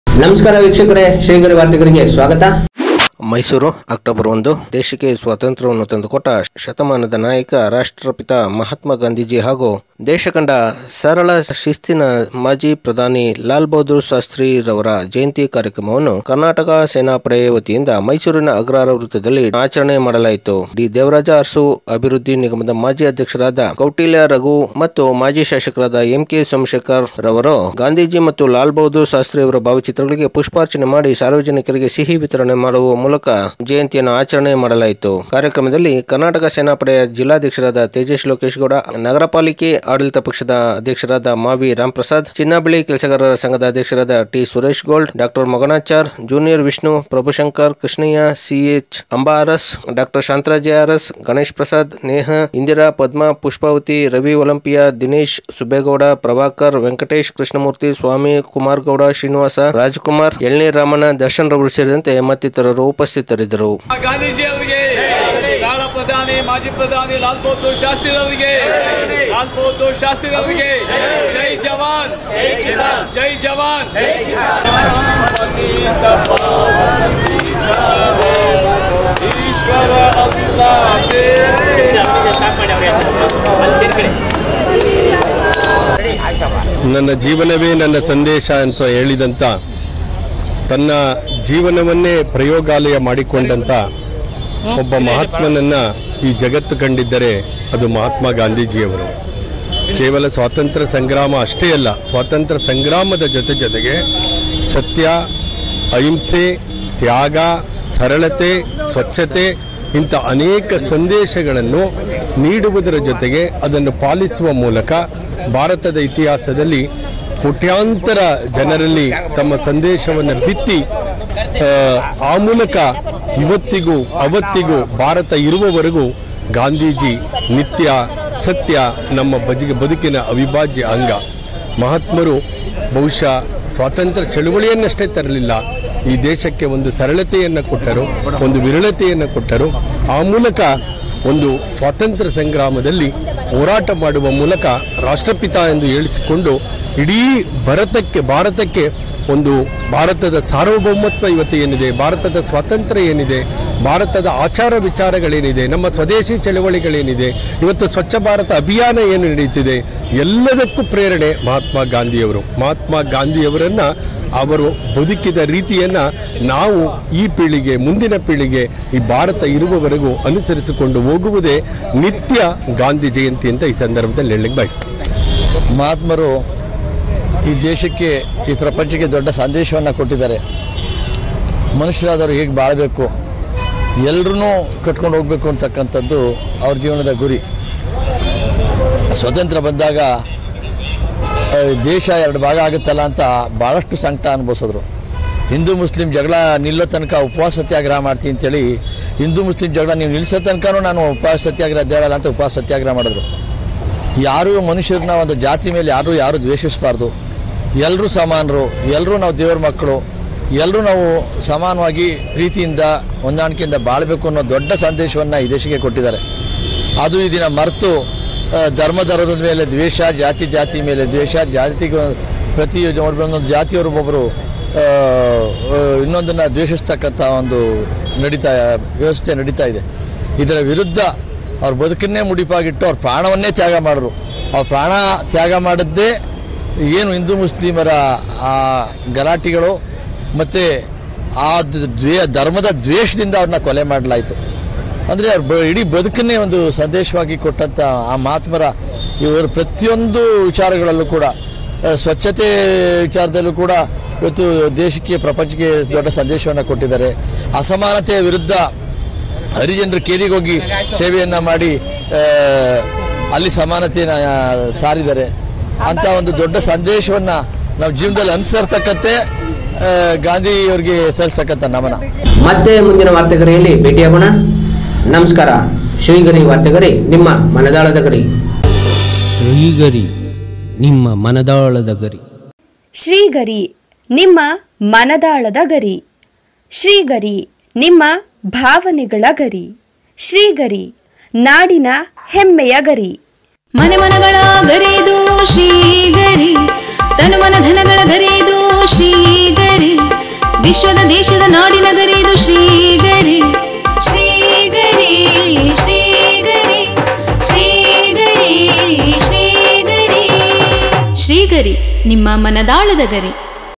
ಕರ್ನಾಟಕ ಸೇನಾಪಡೆ ವತಿಯಿಂದ ಮಹಾತ್ಮ ಗಾಂಧಿ ಹಾಗೂ ಮಾಜಿ ಪ್ರಧಾನಿ ಲಾಲ್ ಬಹದ್ದೂರ್ ಶಾಸ್ತಿç ಜಯಂತಿ....@SHREEGARINEWS
೦೧/೧೦/೨೦೨೩-ಮೈಸೂರು - ಕರ್ನಾಟಕ ಸೇನಾಪಡೆ ವತಿಯಿಂದ ರಾಷ್ಟçಪಿತ ಮಹಾತ್ಮ ಗಾಂಧಿ ಹಾಗೂ ಮಾಜಿ ಪ್ರಧಾನಿ ಲಾಲ್ ಬಹದ್ದೂರ್ ಶಾಸ್ತಿç ಜಯಂತಿ ಆಚರಣೆ.

ಮೈಸೂರು ಅಕ್ಟೊಬರ್ 01

ವಿಶ್ವ ಕಂಡ ಅಪ್ರತಿಮ ಹೋರಾಟಗಾರ,ದೇಶಕ್ಕೆ ಸ್ವಾತಂತ್ರ್ಯವನ್ನು ತಂದುಕೊಟ್ಟ ಶತಮಾನದ ನಾಯಕ, ರಾಷ್ಟ್ರಪಿತ,ಮಹಾತ್ಮ ಗಾಂಧೀಜಿ ಹಾಗೂ ದೇಶ ಕಂಡ ಸರಳ ಶಿಸ್ತಿನ ಮಾಜಿ ಪ್ರಧಾನಿ ಲಾಲ್ ಬಹುದ್ದೂರ್ ಶಾಸ್ತ್ರೀಜಿ ರವರ ಜಯಂತಿ ಕಾರ್ಯಕ್ರಮವನ್ನು ಇಂದು ಕರ್ನಾಟಕ ಸೇನಾ ಪಡೆಯ ವತಿಯಿಂದ ಮೈಸೂರಿನ ಅಗ್ರಹಾರ ವೃತ್ತದಲ್ಲಿ ಡಿ.ದೇವರಾಜು ಅರಸು ಅಭಿವೃದ್ಧಿ ನಿಗಮದ ಮಾಜಿ ಅಧ್ಯಕ್ಷರಾದ ಶ್ರೀ ಕೌಟಿಲ್ಯ ರಘು ರವರು ಮತ್ತು ಮಾಜಿ ಶಾಸಕರಾದ ಎಂ.ಕೆ. ಸೋಮಶೇಖರ್ ರವರು ಗಾಂಧೀಜಿ ಮತ್ತು ಲಾಲ್ ಬಹದ್ದೂರ್ ಶಾಸ್ತ್ರಿ ರವರ ಭಾವಚಿತ್ರಗಳಿಗೆ ಪುಷ್ಪಾರ್ಚನೆ ಮಾಡಿ,ಸಾರ್ವಜನಿಕರಿಗೆ ಸಿಹಿ ವಿತರಣೆ ಮಾಡುವ ಮೂಲಕ ಜಯಂತಿಯನ್ನು ಆಚರಣೆ ಮಾಡಲಾಯಿತ್ತು.

ಪುಟ್ಟ ಮಕ್ಕಳಿಗೆ ಗಾಂಧೀಜಿ ಮತ್ತು ಲಾಲ್ ಬಹದ್ದೂರ್ ಶಾಸ್ತ್ರಿರವರ ವೇಷವನ್ನು ತೊಡಿಸಿ,ವಿಶೇಷವಾಗಿ ಅವರ ಮೂಲಕ ಪುಷ್ಪಾರ್ಚನೆ ಮತ್ತು ಸಿಹಿಯನ್ನು ವಿತರಣೆ ಮಾಡಿದ್ದೂ ವಿಶೇಷವಾಗಿತ್ತು.

ಇನ್ನು ಈ ಕಾರ್ಯಕ್ರಮದಲ್ಲಿ ಕರ್ನಾಟಕ ಸೇನಾ ಪಡೆಯ ಜಿಲ್ಲಾಧ್ಯಕ್ಷರಾದ ತೇಜೇಶ್ ಲೋಕೇಶ್ ಗೌಡ,ಆಡಳಿತ ಪಕ್ಷದ ಅಧ್ಯಕ್ಷರಾದ ಮಾ.ವಿ.ರಾಮ್ ಪ್ರಸಾದ್,ಚಿನ್ನ-ಬೆಳ್ಳಿ ಕೆಲಸಗಾರರ ಸಂಘದ ಅಧ್ಯಕ್ಷರಾದ ಟಿ.ಸುರೇಶ ಗೋಲ್ಡ್,ಡಾ.ಮೊಗ್ಗಣ್ಣಚಾರ್,